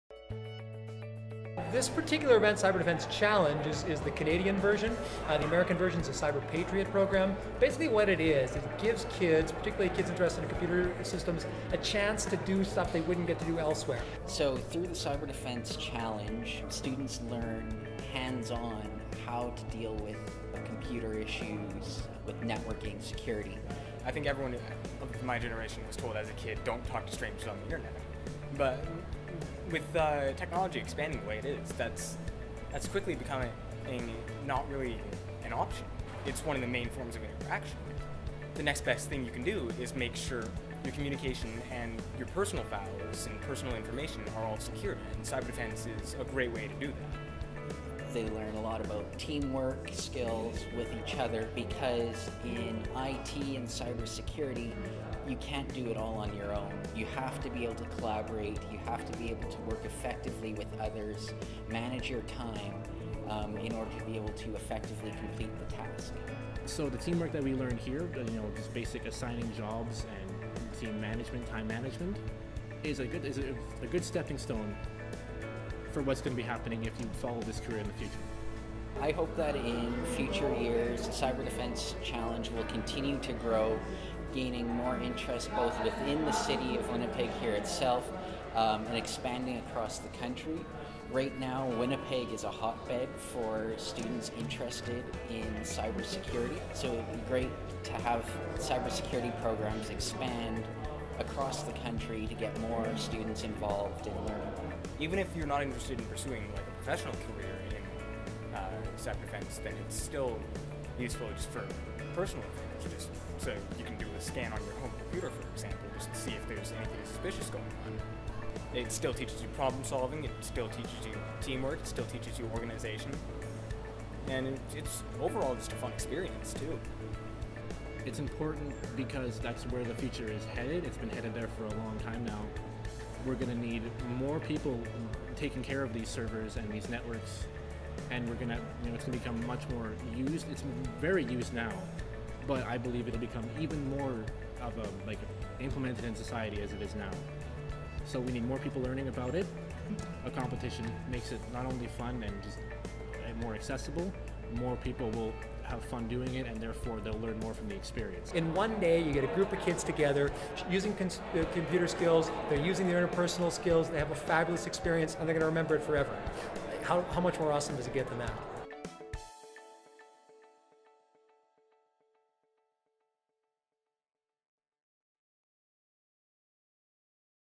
LRSD at the Canadian Cyber Defence Challenge 2016
See how students in the Louis Riel School Division are using their coding skills at the 2016 Canadian Cyber Defence Challenge.